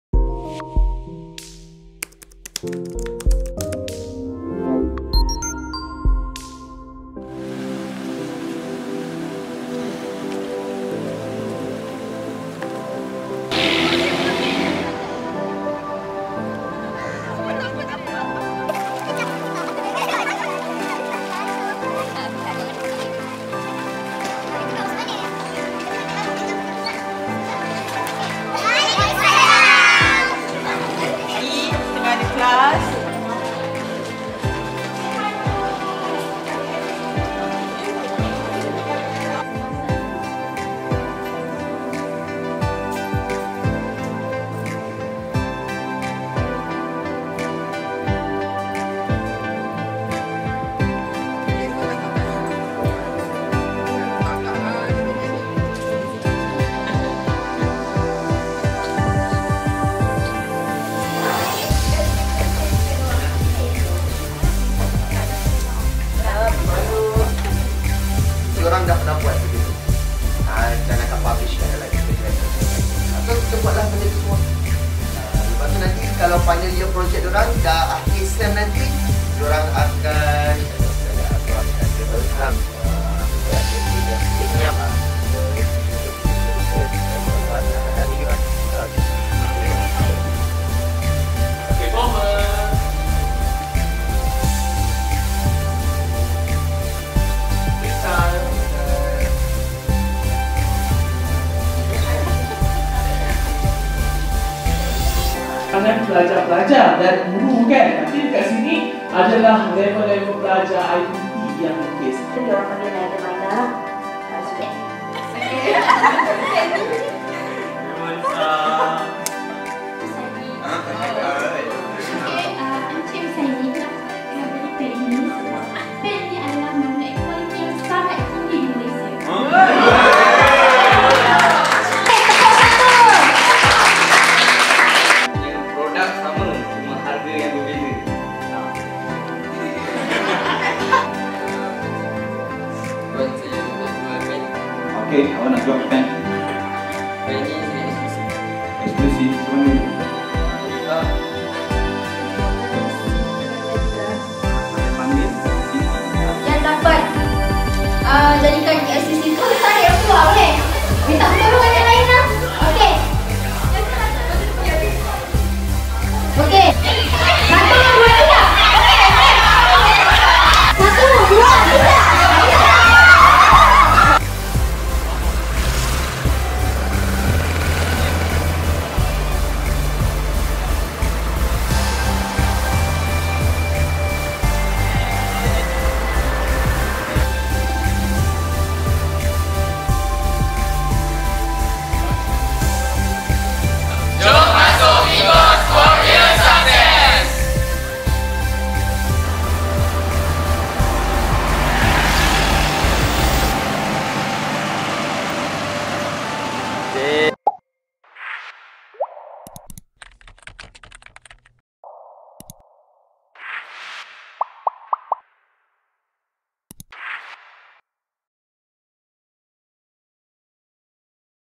MICOST Campus Tour Graha Maju 2020
Melaka International College of Science and Technology (MiCoST) is a private institution of higher learning wholly-owned by the Melaka State Foundation (Yayasan Melaka) under its subsidiary company, YM Mediscience Sdn Bhd. MiCoST is located at the 6th Floor Bangunan Graha Maju, Jalan Hang Tuah, 75300 Melaka.